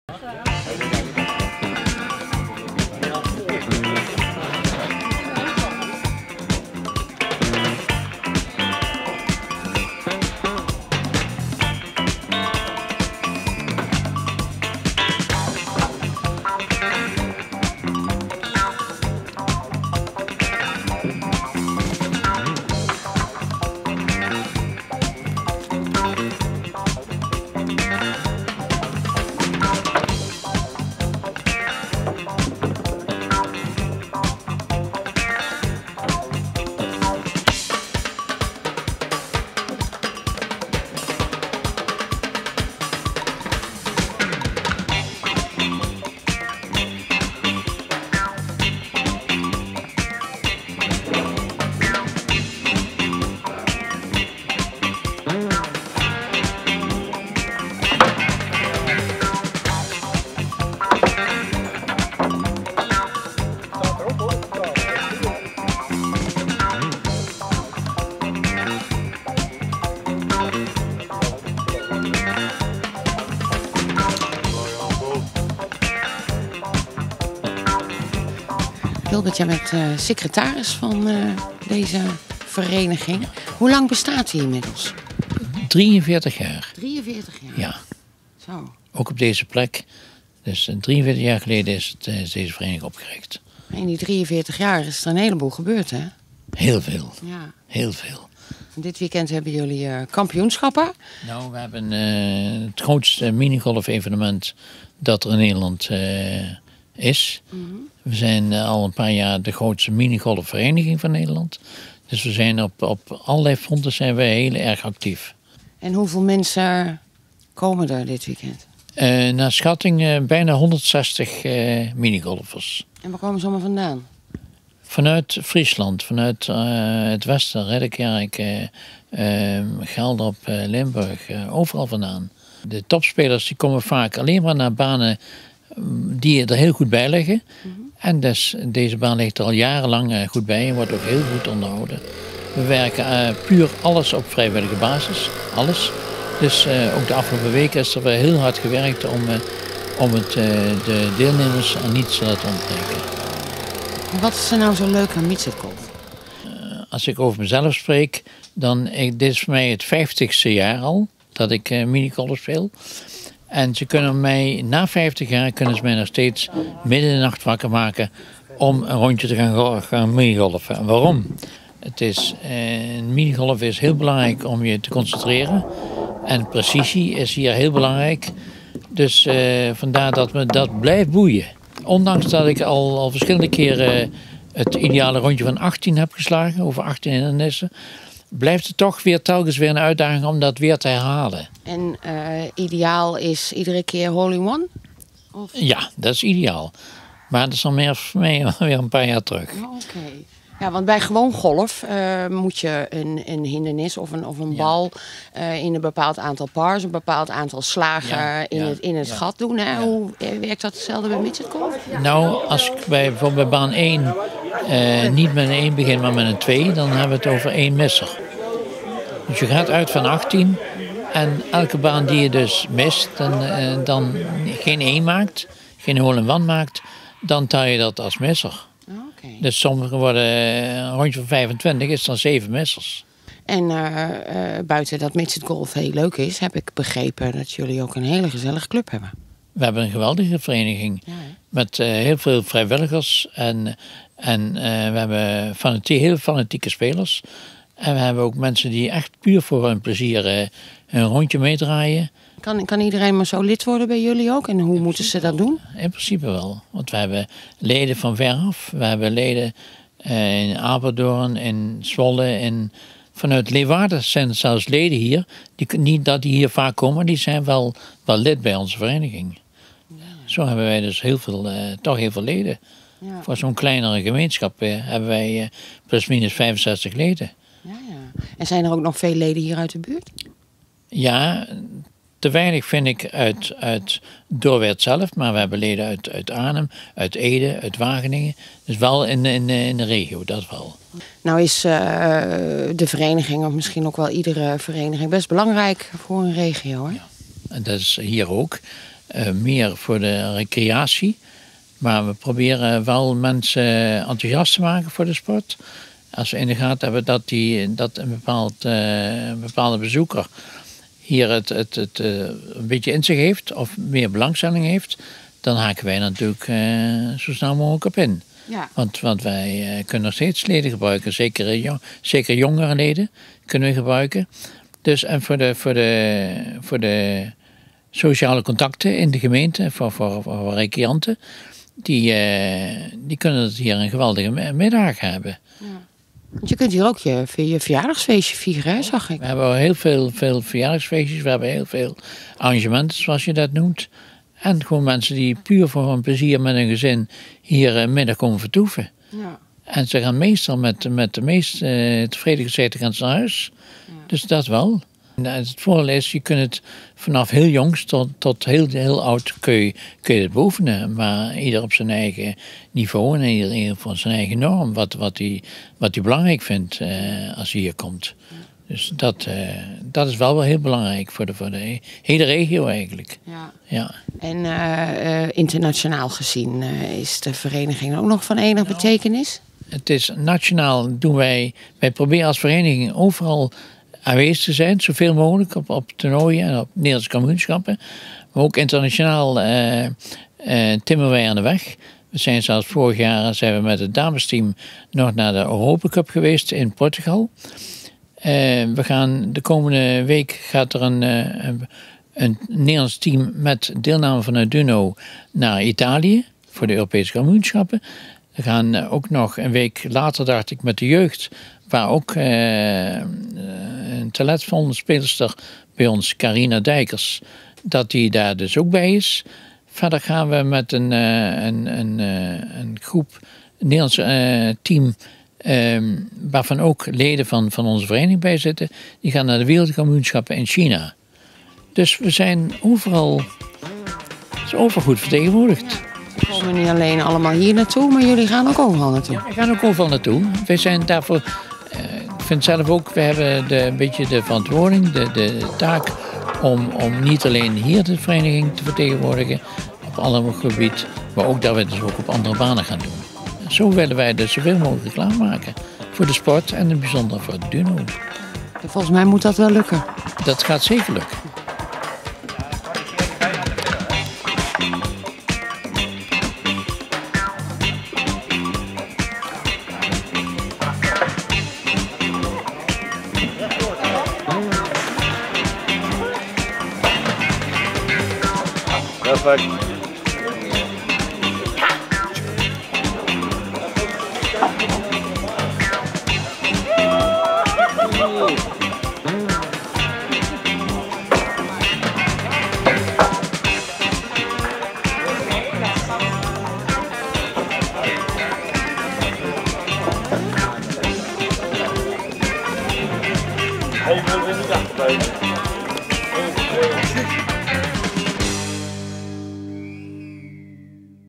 MGV DUNO
Sfeerimpressie Duno-toernooi en gesprek met secretaris Wilbert van Helmond